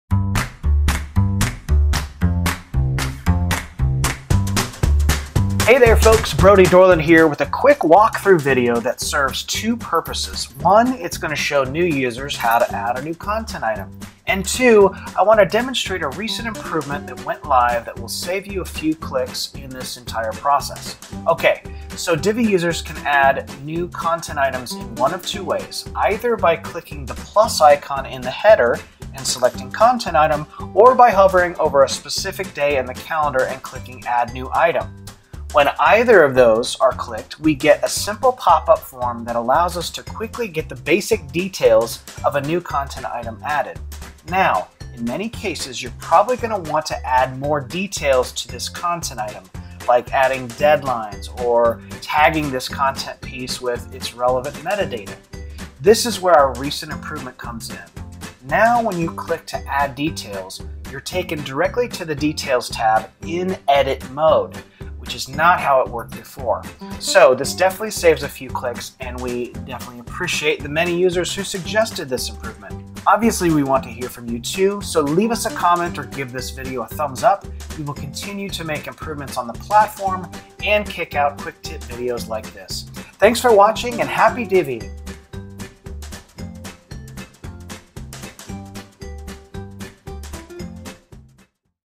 Adding New Content Items & Details (IMPROVED) in DivvyHQ
- Hey there folks, Brody Dorland here with a quick walkthrough video that serves two purposes...

1) It will show new users how to add a new content item, and
2) I want to demonstrate a recent improvement that went live that will save you a few clicks in this process.

Ok, so Divvy users can add new content items in one of two ways, either by clicking the plus icon in the header and selecting Content Item, or by hovering over a specific day in the calendar and clicking Add New Item.

When either of those are clicked, we get a simple pop-up form that allows us to quickly get the basic details of a new content item added. Now, in many cases, you’re probably going to want to add more details to this content item, like adding deadlines or tagging this content piece with its relevant meta data.

This is where our recent improvement comes in. Now when you click to Add Details, you’re taken directly to the details tab -IN EDIT MODE- which is not how it worked before.

So, this definitely saves a few clicks and we definitely appreciate the many users who suggested this improvement. Obviously we want to hear from you too, so leave us a comment or give this video a thumbs up and we will continue to make improvements on the platform and kick out quick tip videos like this.